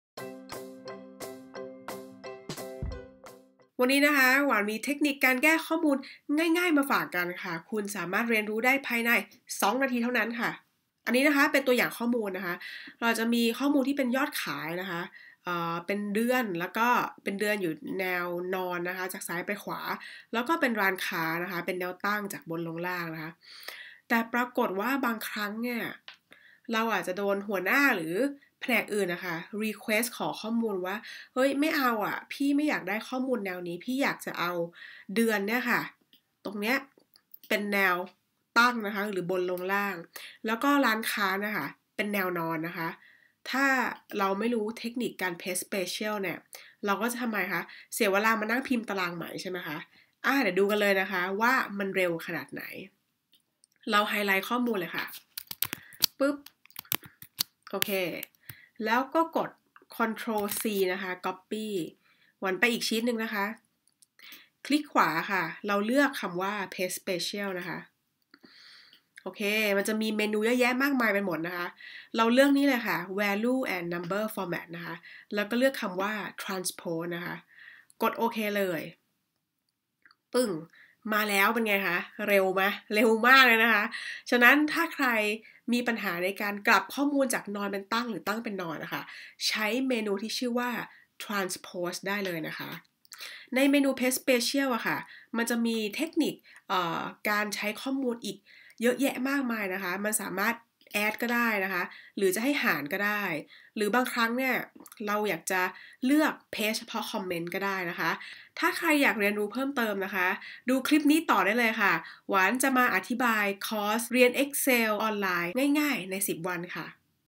สอนการใช้ TRANSPOSE ใน EXCEL การสลับข้อมูลจากแนวนอนเป็นแนวตั้งโดยไม่ต้องพิมพ์ใหม่
สอนการใช้ TRANSPOSE ใน EXCEL

การสลับข้อมูลจากแนวนอนเป็นแนวตั้งโดยไม่ต้องพิมพ์ใหม่

เพื่อนๆคนไหนเคยที่ต้องนั่งทำข้อมูลใหม่ใน Excel บ้าง

เพียงแค่ต้องสลับข้อมูลจาก

เราสามารถทำได้โดยไม่ต้องพิมพ์ใหม่นะคะ นั่นก็คือใช้ การ Transpose ซึ่งง่ายๆ มากๆ Click ดูใน VDO เล้ยย!!

EXCEL TRANSPOSE